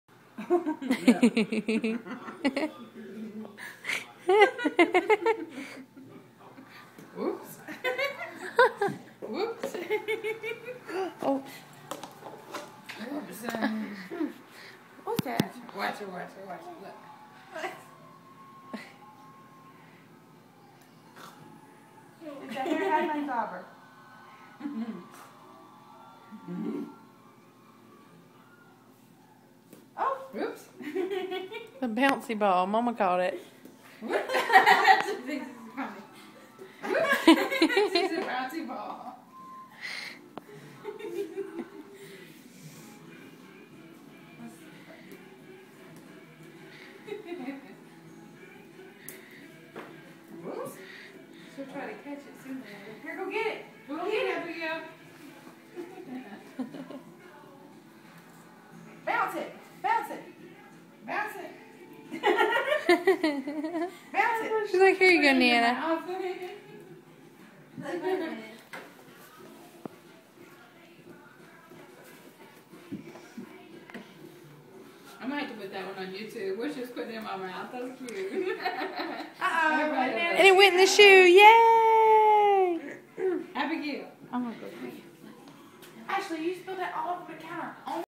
iphone pic 10 27 2011 006.MOV
Me and my granddaughter Abigail playing with a ball, she is so silly